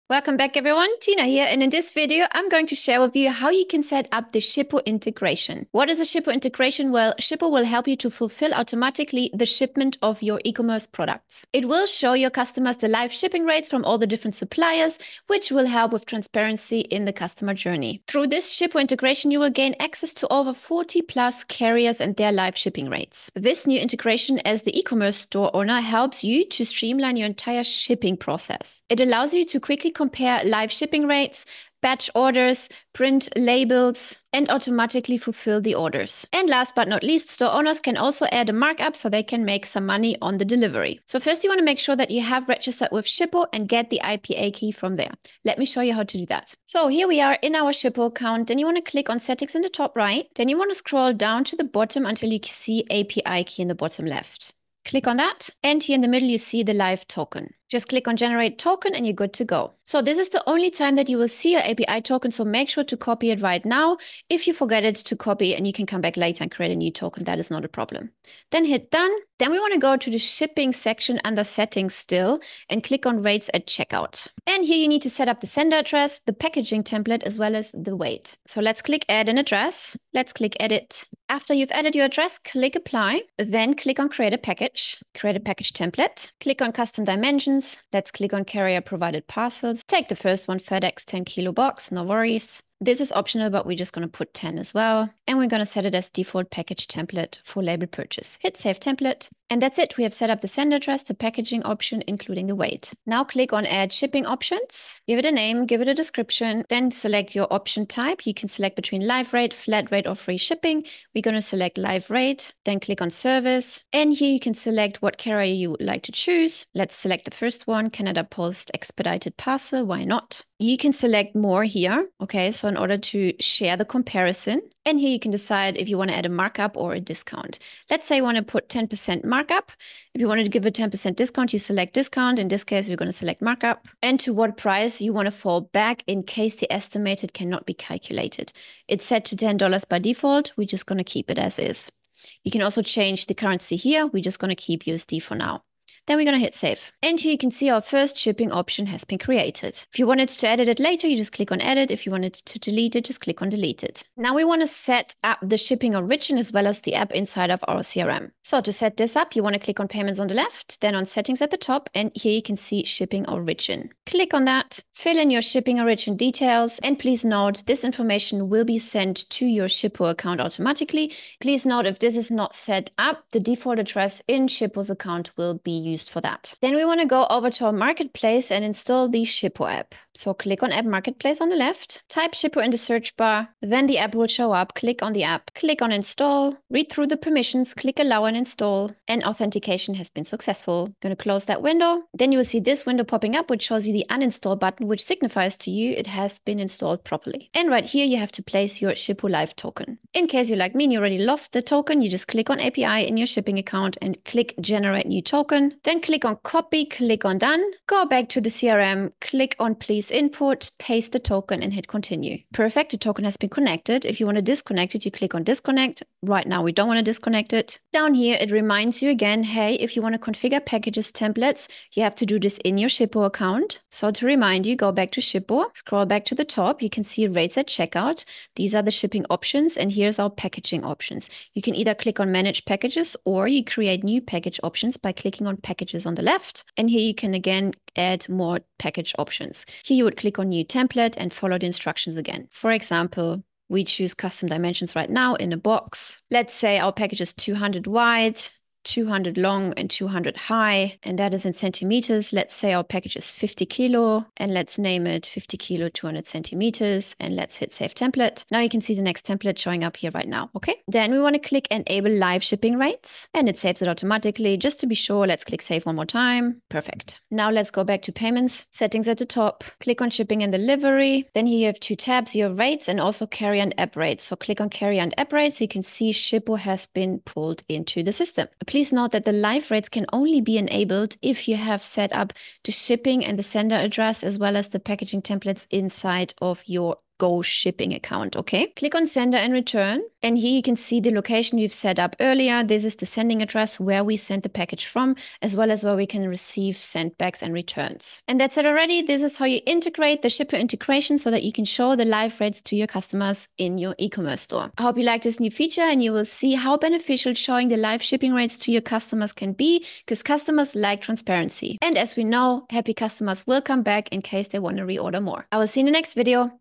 Shippo Integration For E-Com Stores LIVE on Gohighlevel: Automatic Fulfillment & Live Shipping Rates
🚀 Any automation of your choice built for free